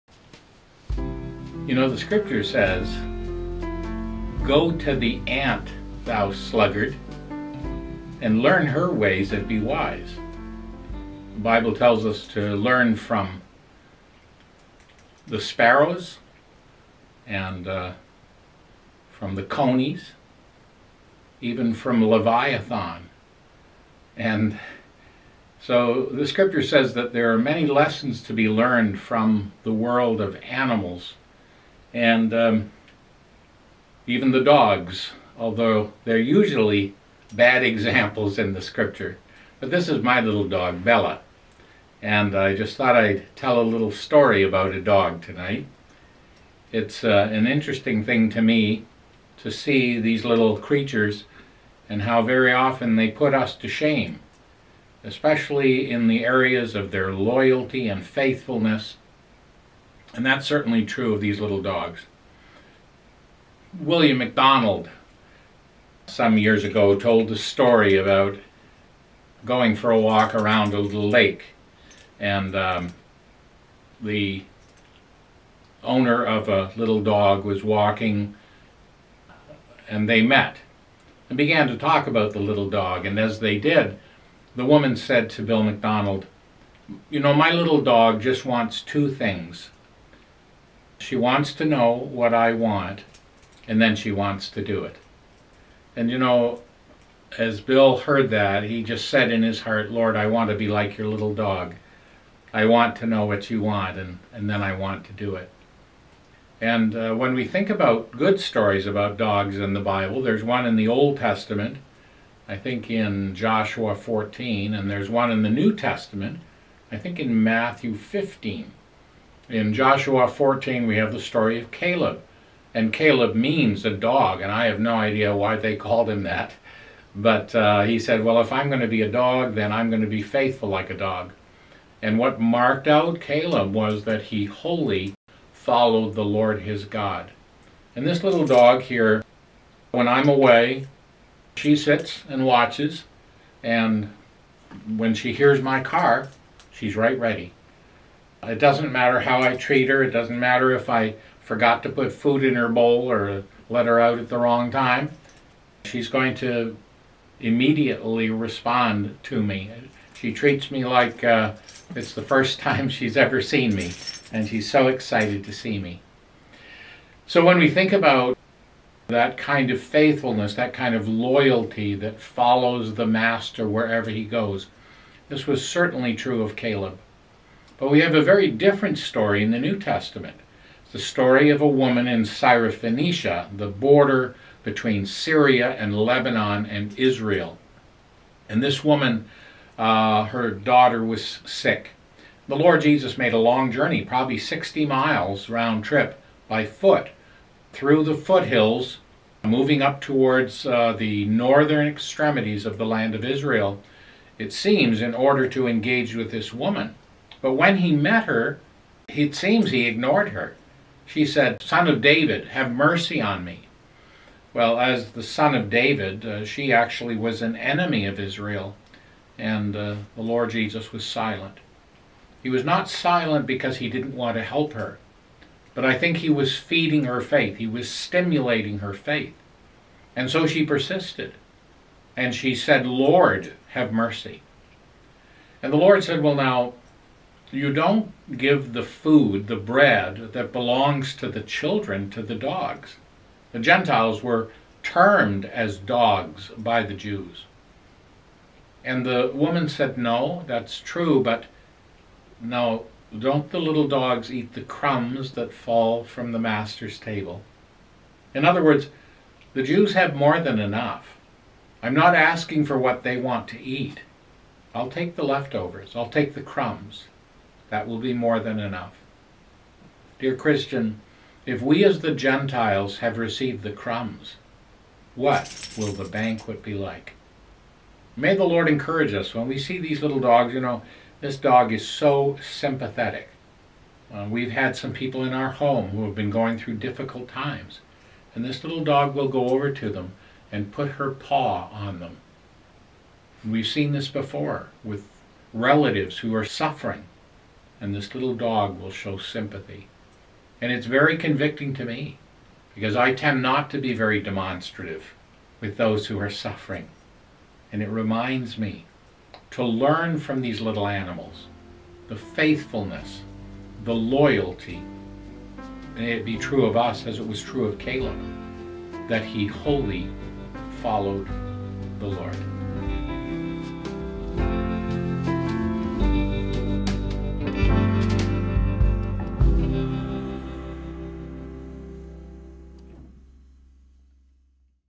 Fireside Friday: Look Who's Gone to the Dogs
Meet Bella, my only family member with four legs. In most cases, dogs don't fare well in Bible stories, but there are a few inspiring exceptions.